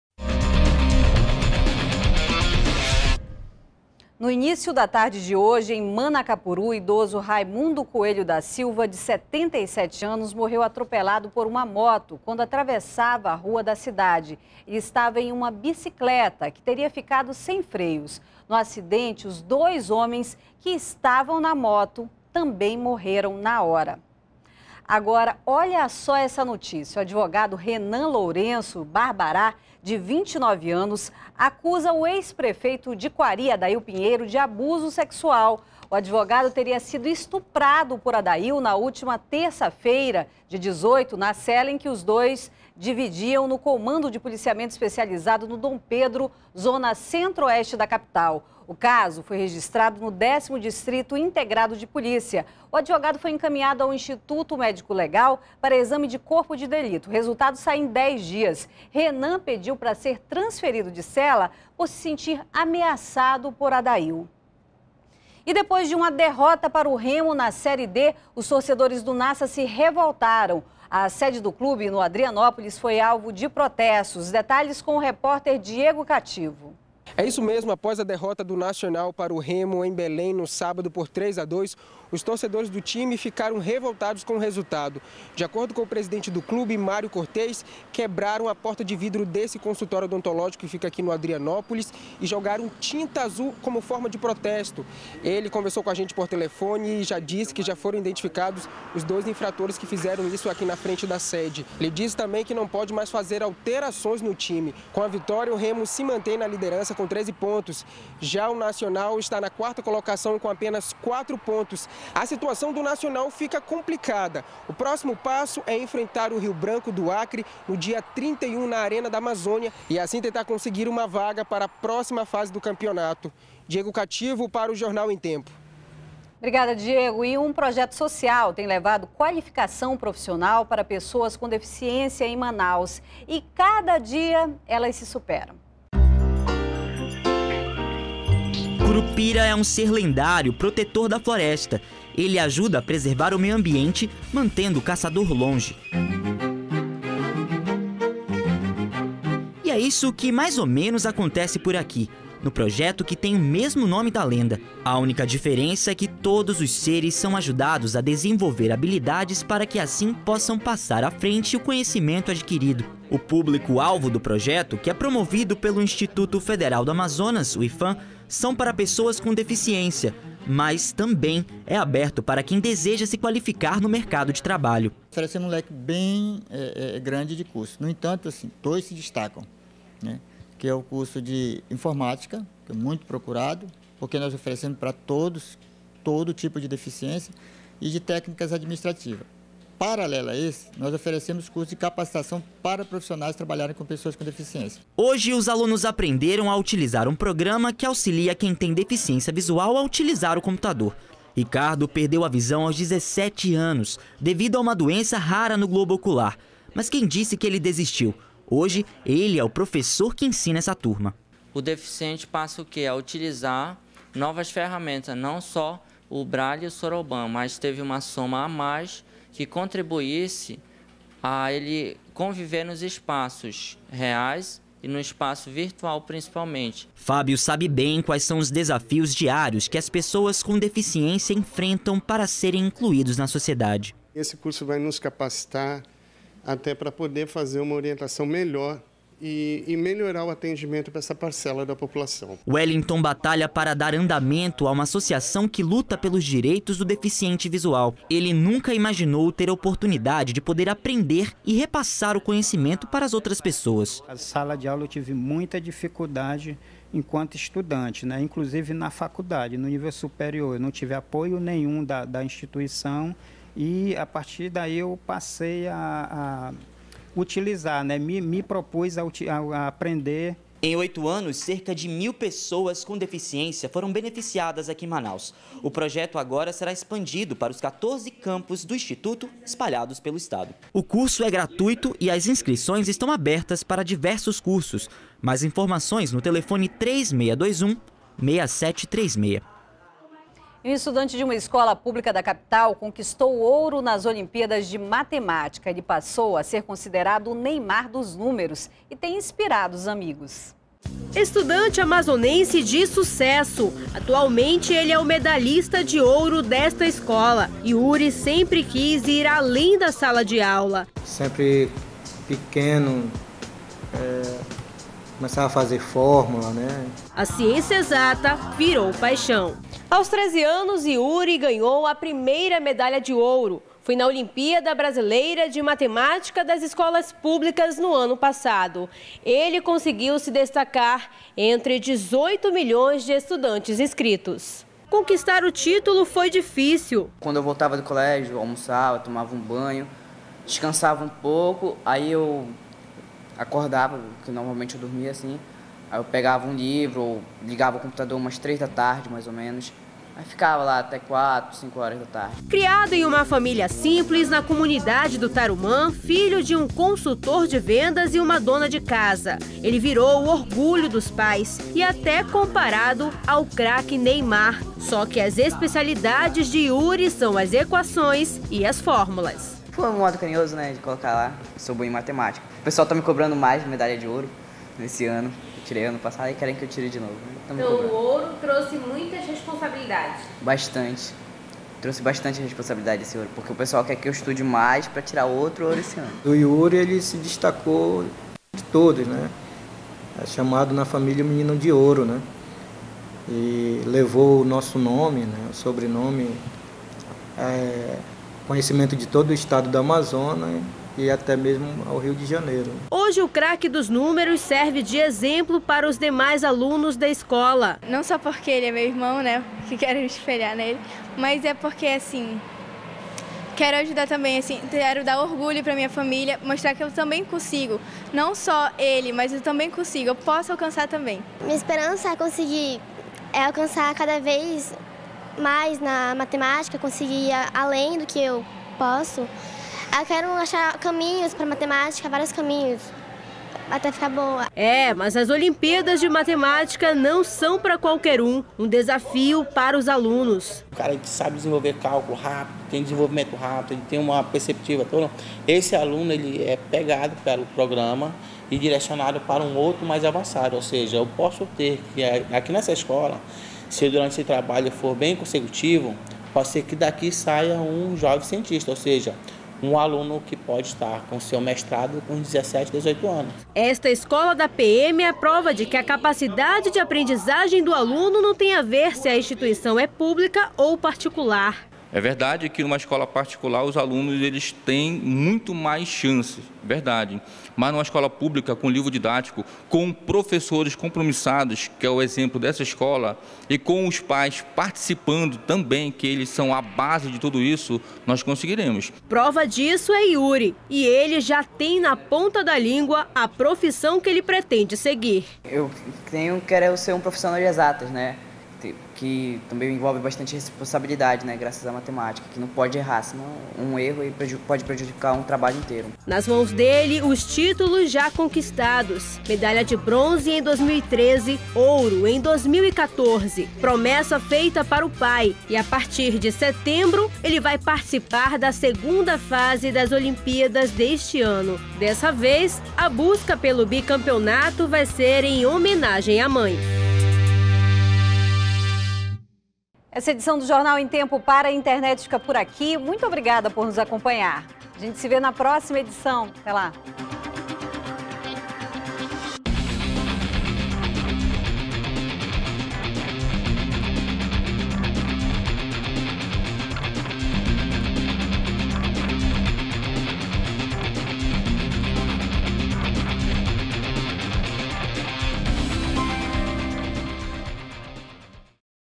Estudante de uma escola pública de Manaus conquista ouro nas Olimpíadas de matemática
03º BLOCO JORNAL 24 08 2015 - Estudante de uma escola pública de Manaus conquista ouro nas Olimpíadas de matemática